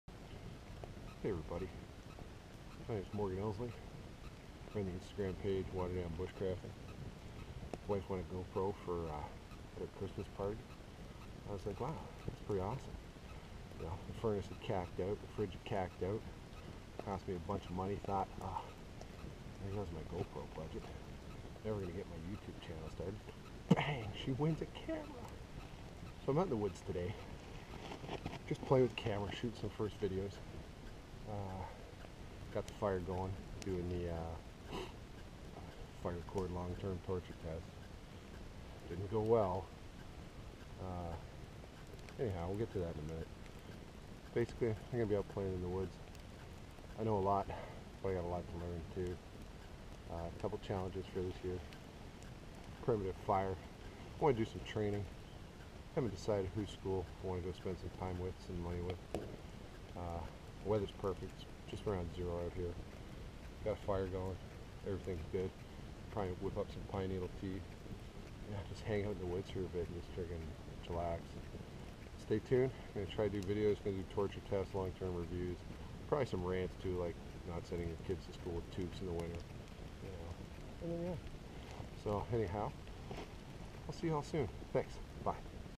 Bushcraft - #1. Intro.
My first Video.  Out and about today. Playing with the camera, Learning what does and doesn't work with the Go pro..A couple thoughts and some good fun in the woods!